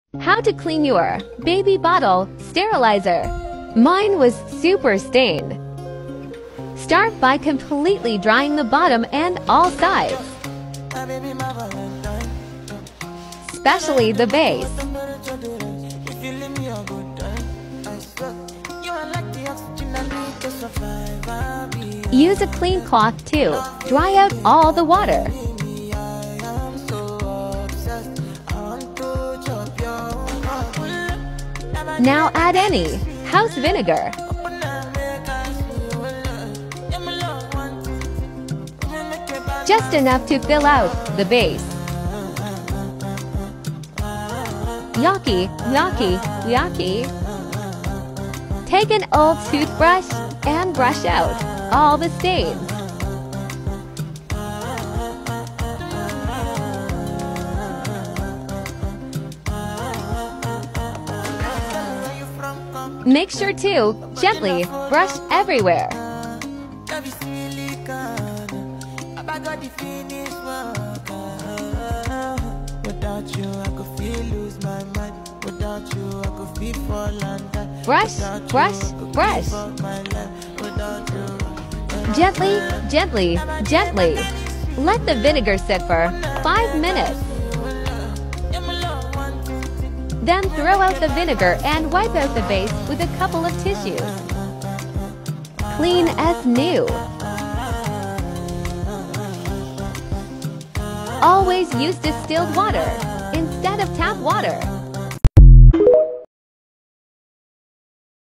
How To Clean Baby Bottle Sterilizer - Removing Stains
To avoid future stains, always use distilled water instead of tap water when using the sterilizer